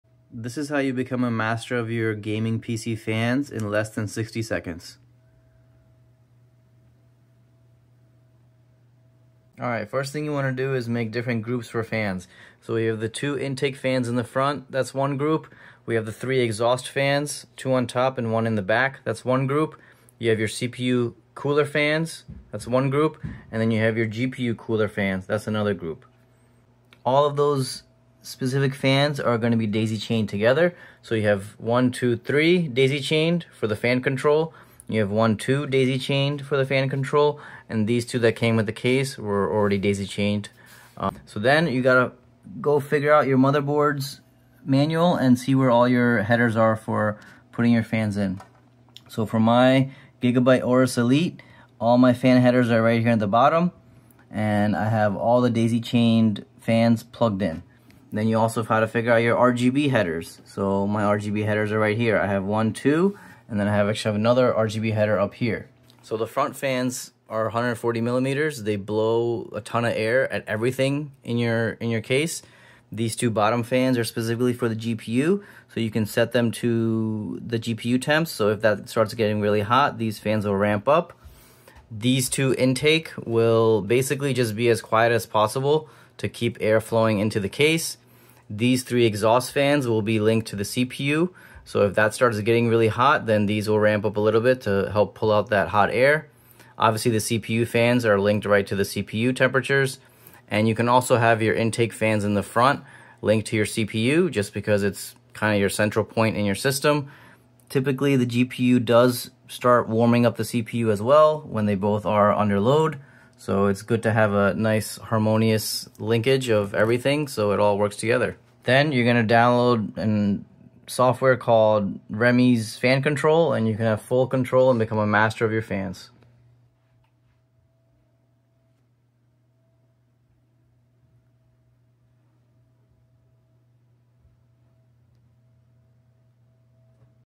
How to setup and control your PC fans for best cooling
How to setup your fans in your PC for optimal cooling and ultra quiet running. I use 2 large 140mm intake fans at the front of the case and 2 smaller 120mm intake fans under the Nvidia rtx 4070 super GPU. 3 smaller 120mm fans used for exhaust on top and back of the case. And finally the 2 fans on the amd ryzen 7 9800x3d CPU cooler. Each group of fans are daisy chained and connect to individual headers on the motherboard so each group can be linked to the CPU or GPU temps and individually controlled.